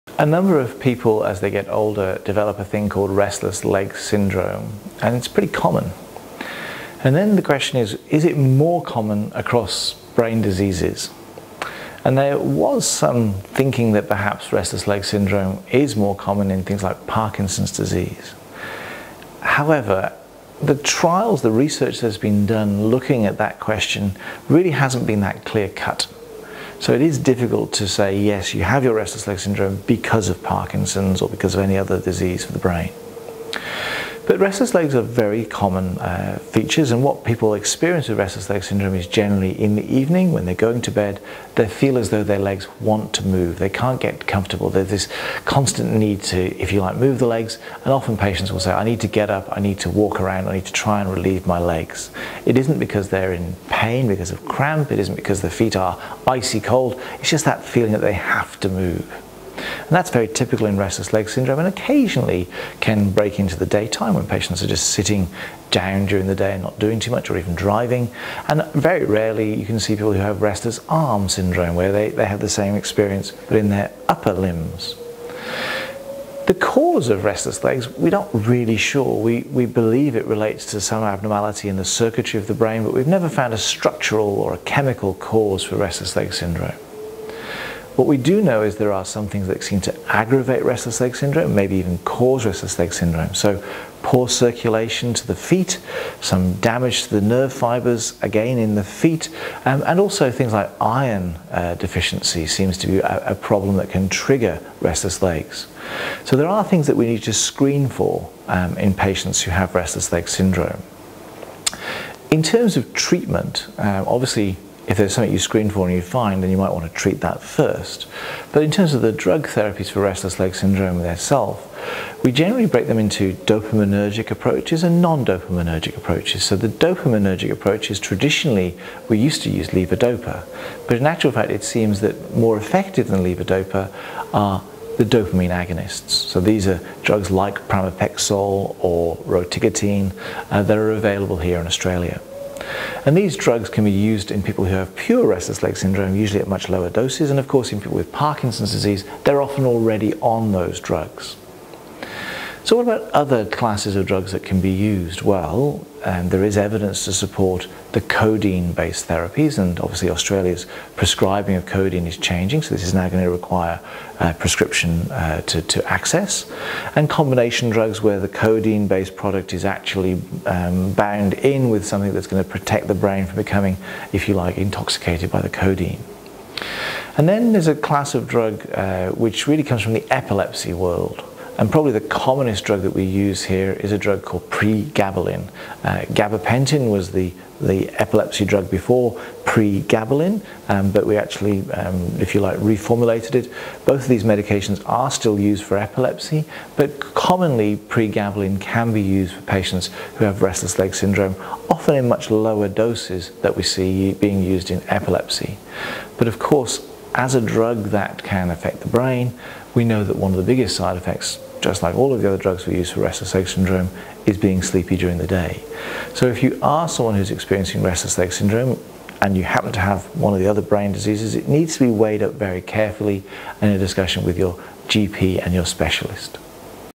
Restless Legs Syndrome
Discover the features, associations and treatments for restless legs syndrome.